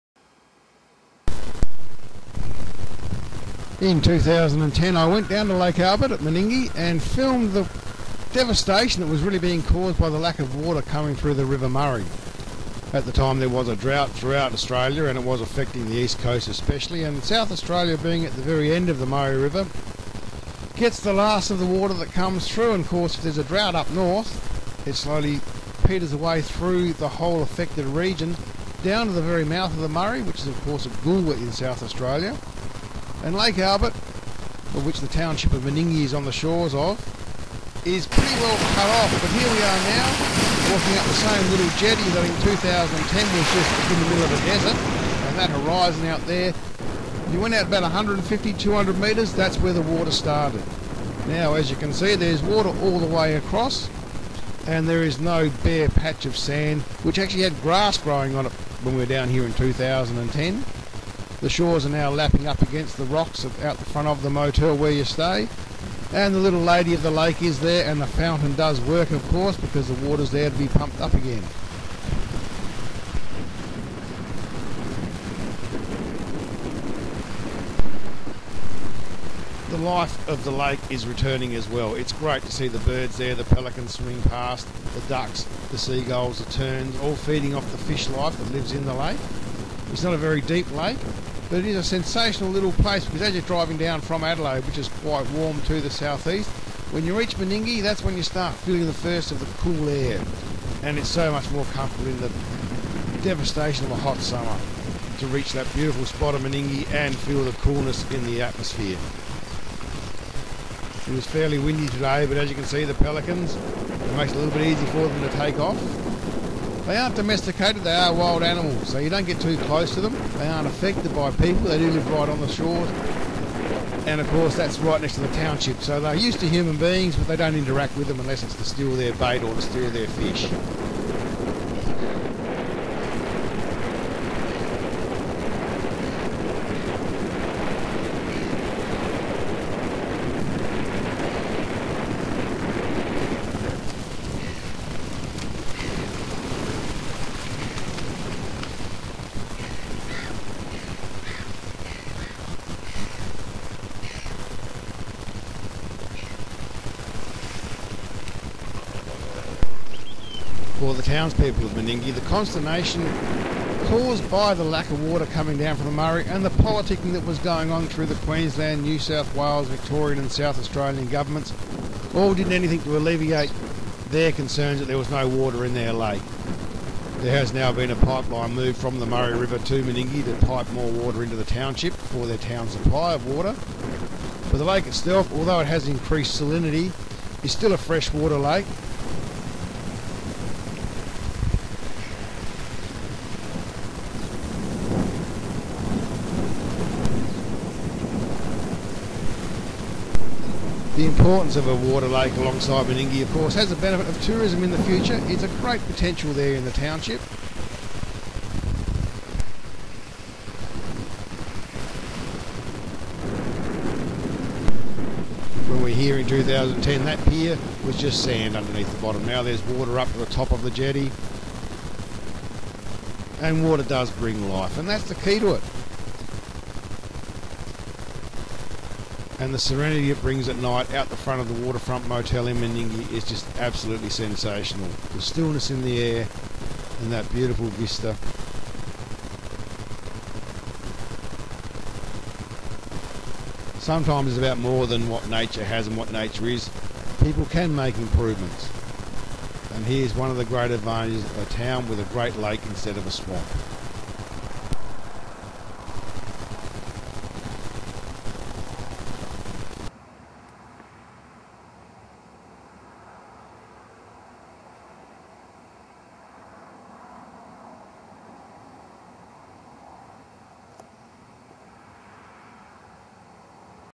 Lake Albert Meningie SA 2013
In 2010 I filmed Lake Albert at Meningie in South Australia which was severely affected bythe drought. As well as the politicking between the states of Queensland, NSW, Victoria and South Australia on water flows down the Murray River. Since then the drought has broken and this is the follow up video that many viewers have requested.